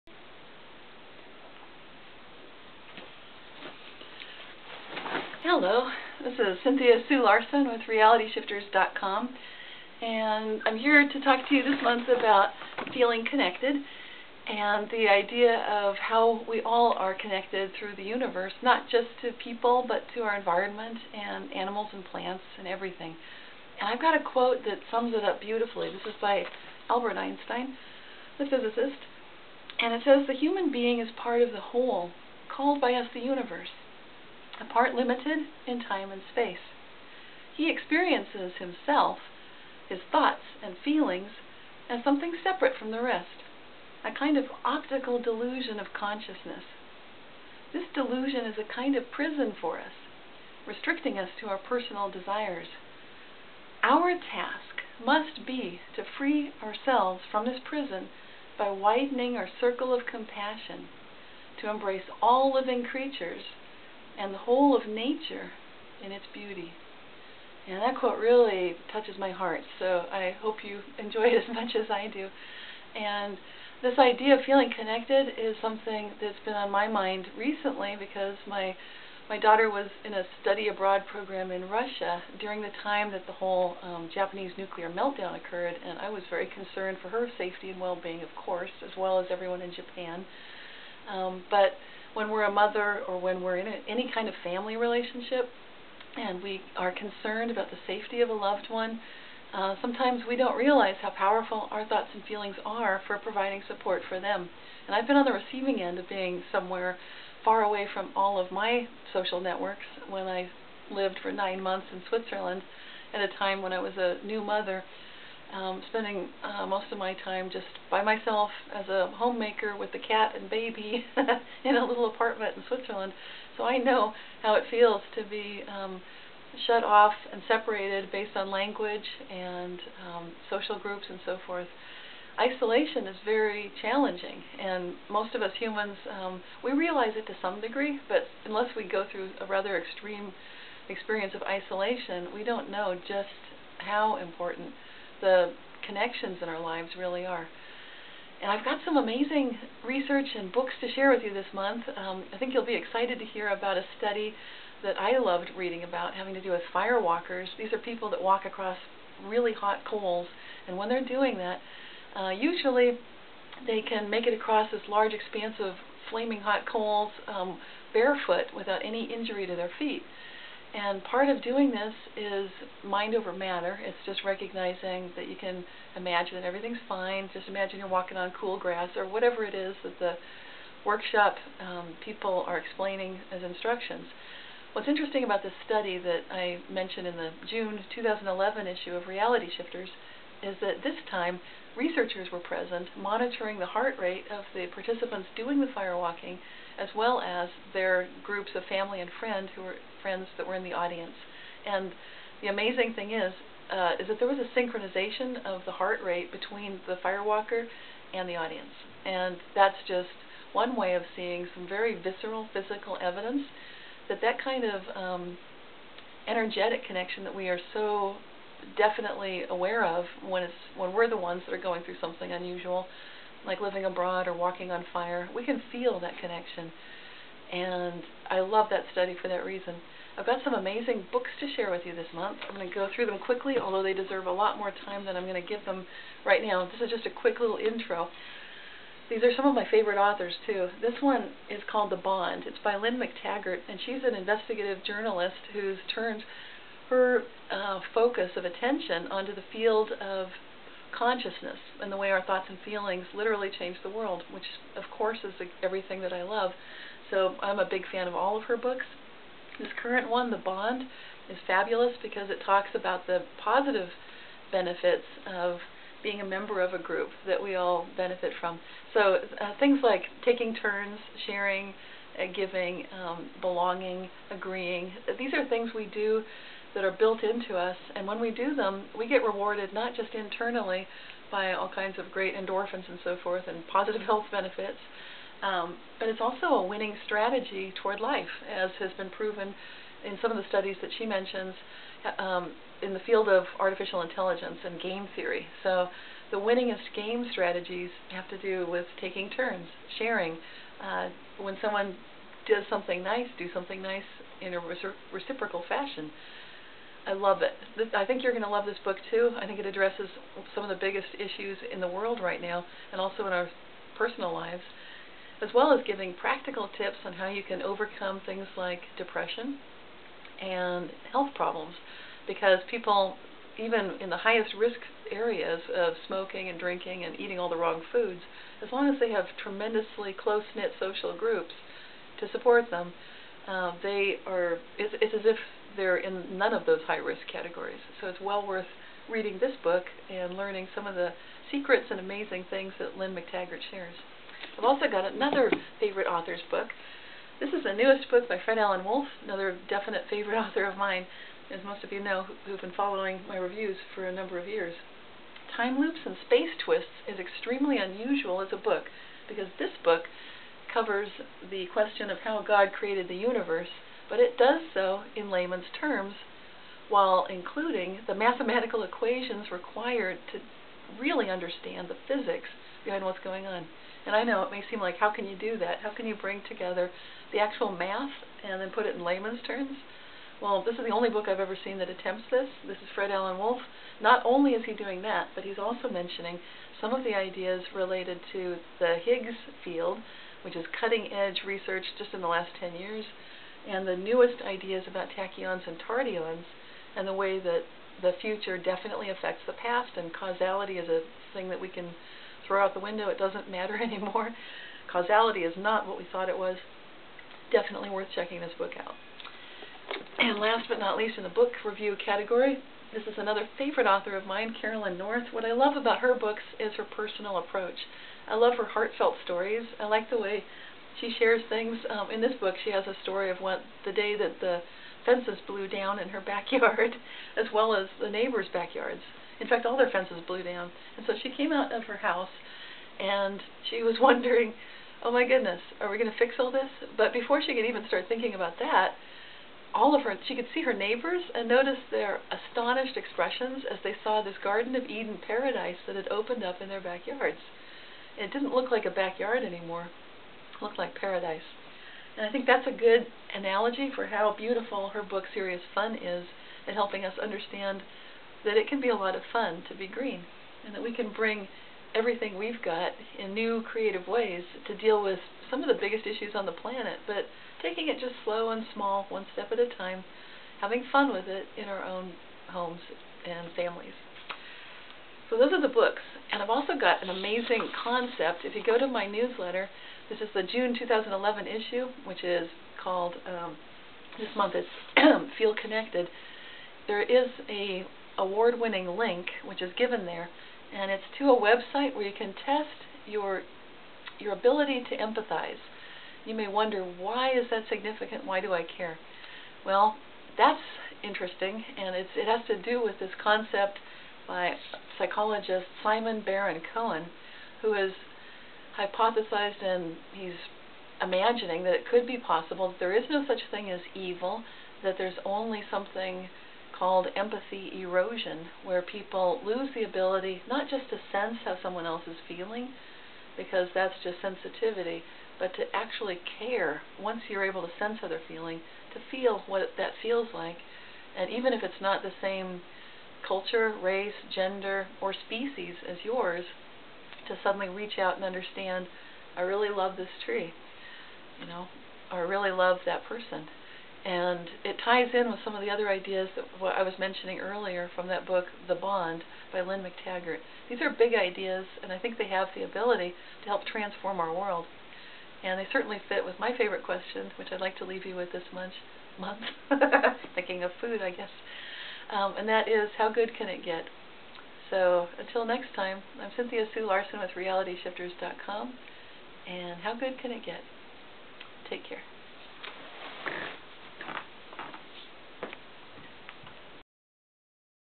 How to Feel Connected
What's one of the most important things to our health and well-being? Cynthia Sue Larson talks about the importance of feeling connected as described in Lynne McTaggart's book, "The Bond." Cynthia shares personal experiences with feeling connected and isolated, and some amazing new research findings from a recent research study of fire walkers. Cynthia reviews Fred Alan Wolf's new book, "Time Loops and Space Twists," and Carolyn North's new book, "Serious Fun," and talks about how you can test your empathy level.  This is a video summary of highlights from the June 2011 issue of RealityShifters which is viewable in its entirety at